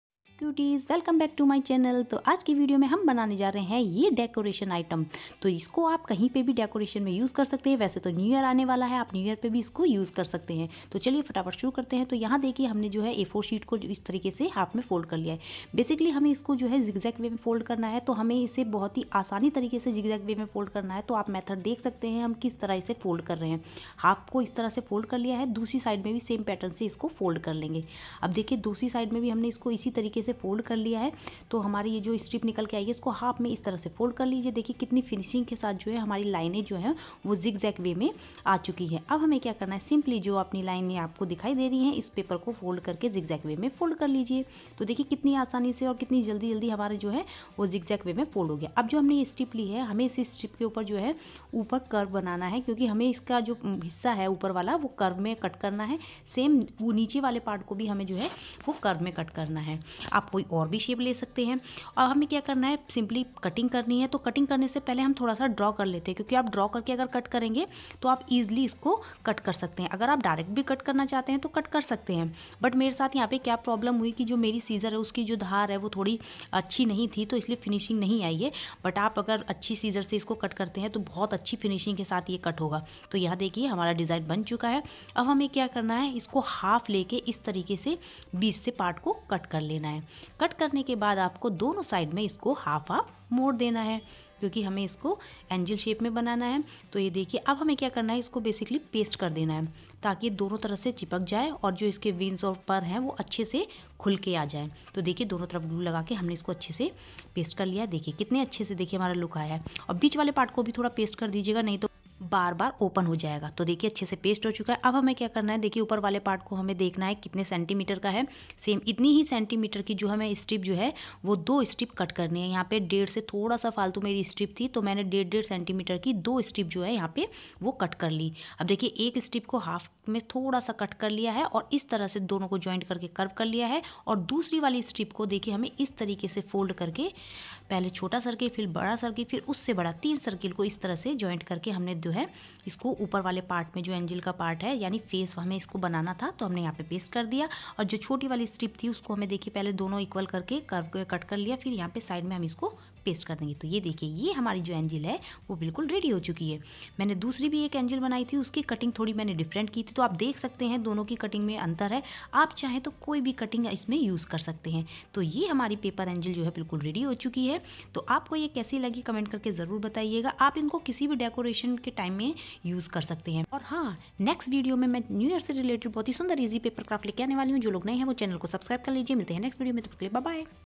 New Year🎇 Decoration idea😇 2024 With A4 Sheet l DIY Easy Paper Craft Angel for New year Decoration
How to make New Year 2024 craft | DIY Craft | Handmade Decoration | New Year Crafts

This video is about How to make New Year 2024 craft | DIY | New Year Crafts. This craft is very easy to make using paper. You can use handmade decoration for new year crafts. This craft is best for new year 2024. Hope you enjoy this video. If you liked our craft ideas, then give us your feedback in comments. Thanks!